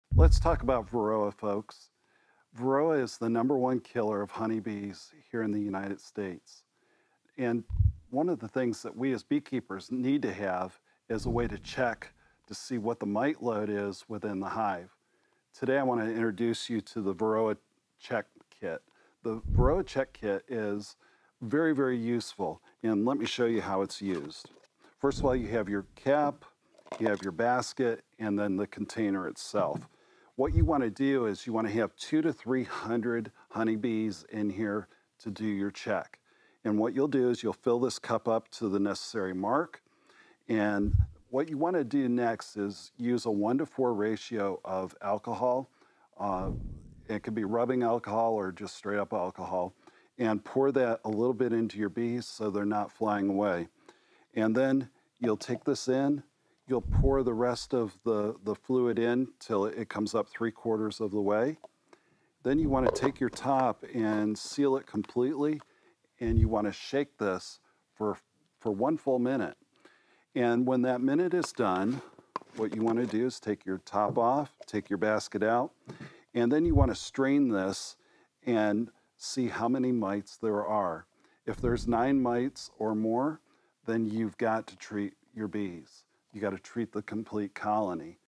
Do You Have a Varroa Mite Problem? Here’s How to Know
Let’s talk about Varroa destructor, folks — the #1 killer of honey bees in the U.S. If you're keeping bees, whether it's one hive or a hundred, you've got to stay on top of your mite levels. Knowing your infestation rate is the first step toward protecting your colonies.

👨‍🔬 One simple and reliable method? The Varroa Check Kit.

Here's how it works:

Collect 200–300 bees from the brood nest (not the entrance).

Pour in rubbing alcohol or windshield washer fluid — a 1:4 ratio is key.

Shake it like you mean it — for a full minute.

Count the mites. If you see 9 or more mites, it’s time to treat. No delay.

The kit includes a basket, container, and lid — easy to use and clean. This isn’t just for pros — it’s for every beekeeper who wants to keep their bees alive and productive.

🧠 Remember: routine sampling (every month during the active season) can save your hives and your honey harvest.

In Ray’s experience, the best beekeepers are the ones who stay curious, stay educated, and always test — never guess.

🐝 Let’s beat the mite, not lose the fight.